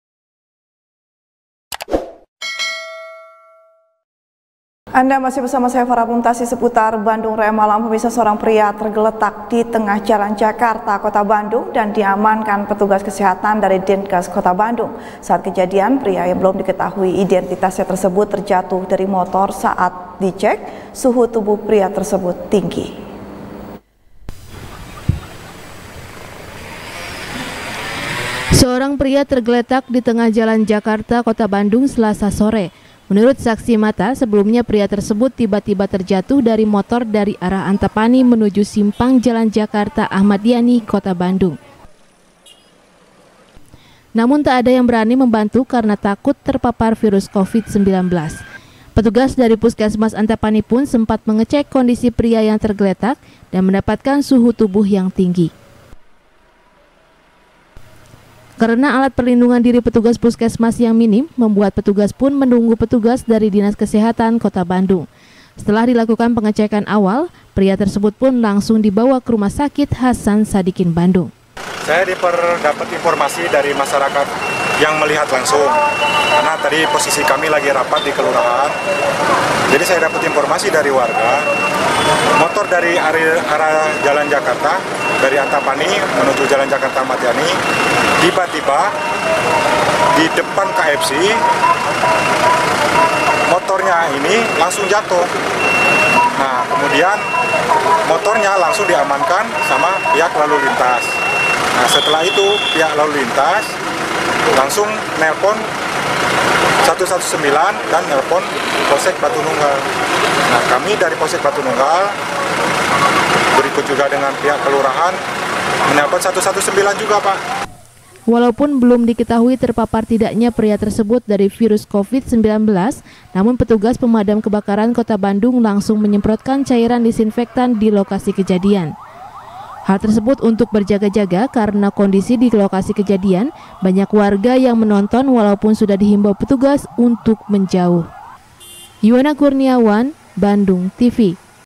Diduga Terpapar Corona, Pria Tergeletak Dievakuasi | SBR | BANDUNGTV
SBR | BANDUNGTV | FARHAT MUMTAZ | Seorang Pria Tergeletak Di Tengah Jalan Jakarta, Kota Bandung, Dan Diamankan Petugas Kesehatan Dari Dinkes Kota Bandung. Saat Kejadian, Pria Yang Belum Diketahui Identitasnya Tersebut, Terjatuh Dari Motor, Dan Saat Dicek, Suhu Tubuh Pria Tersebut, Tinggi.